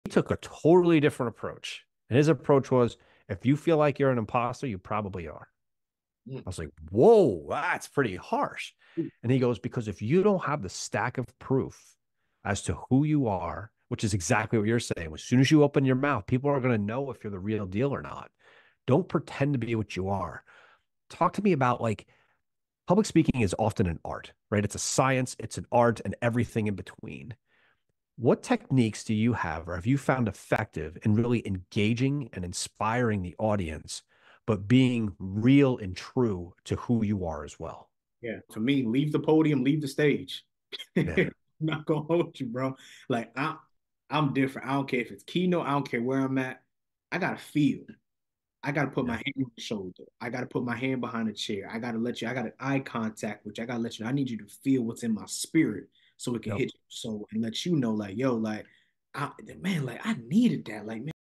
Unlocking the Art of Engaging Public Speaking | Techniques for Inspiring Authenticity
Discover effective techniques to engage and inspire your audience while staying true to yourself. Learn how to unlock the art of public speaking with authenticity and leave a lasting impact.